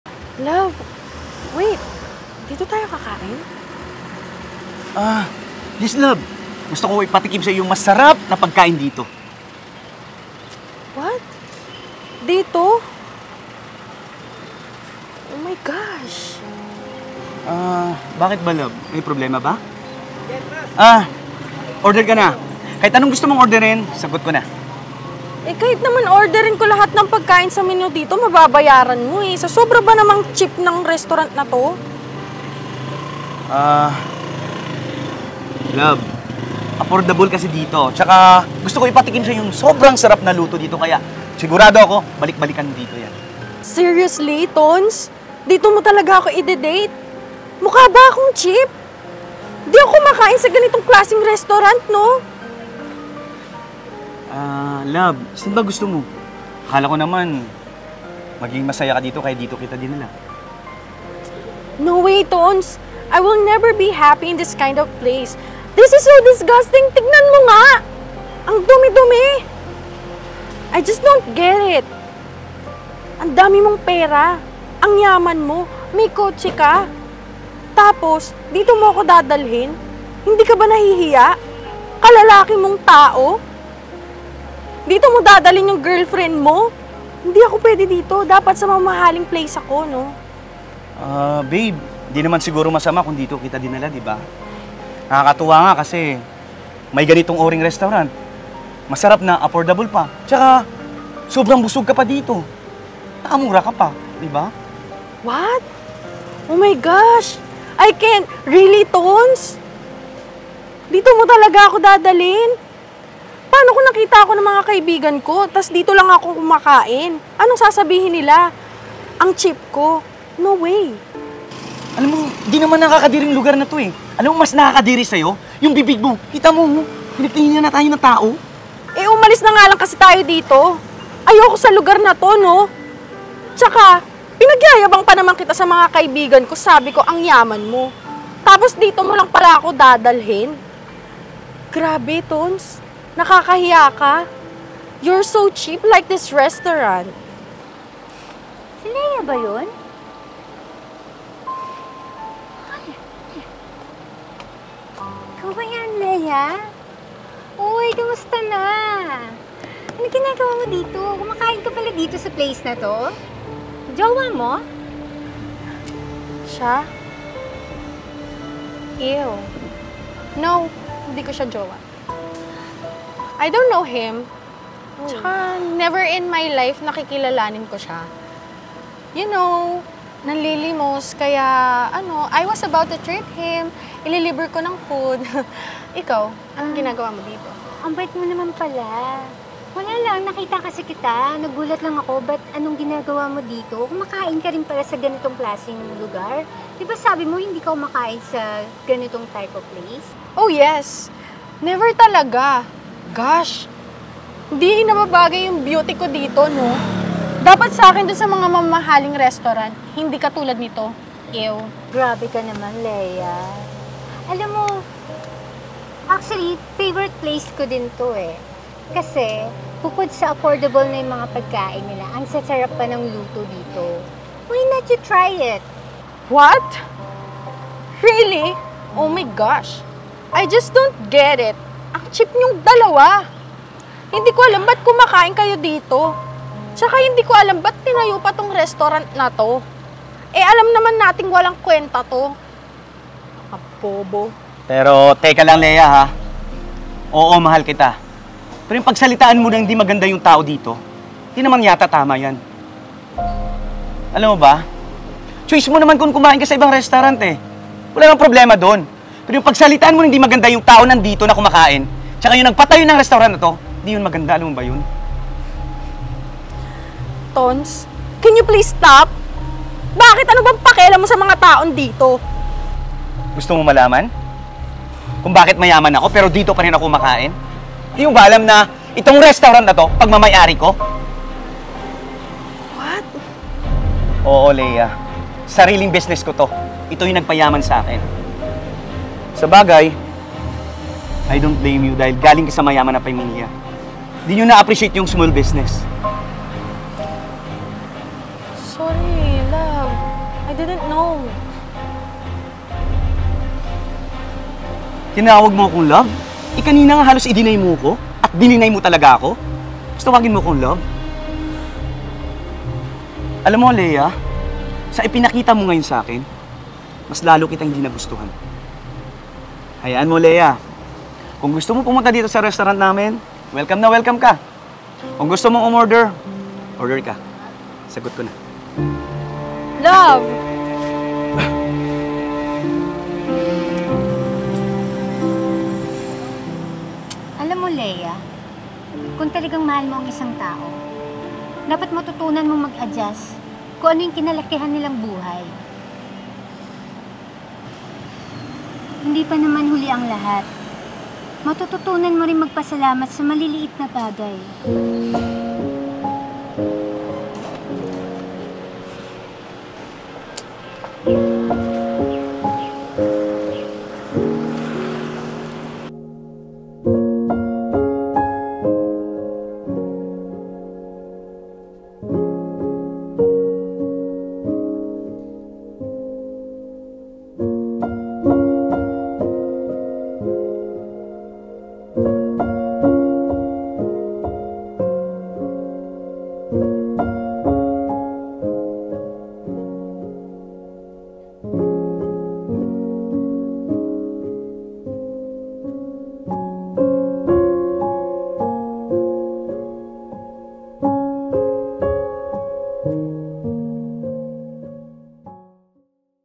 Cheapipay Girlfriend.
A short film about a narcissist girlfriend.